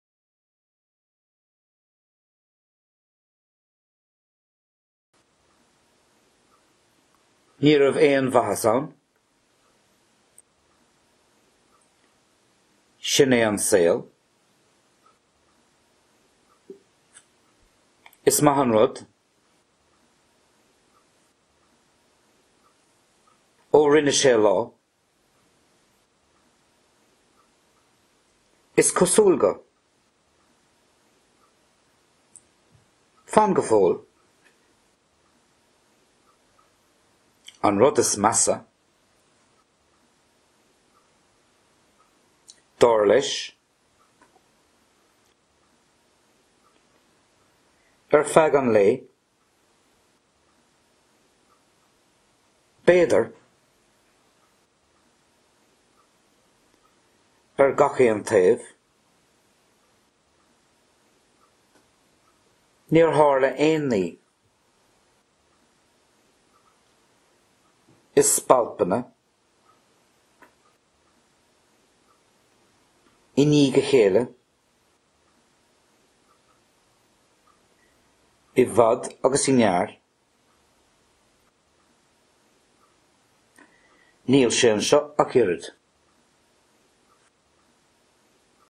Nathanna Cainte 9
This is a "leabhar cainte" i.e a talking Irish book. These are useful phrases to enhance your written and spoken Irish. Must be viewed in HD! Bain taitneamh as! . Míle Buíochas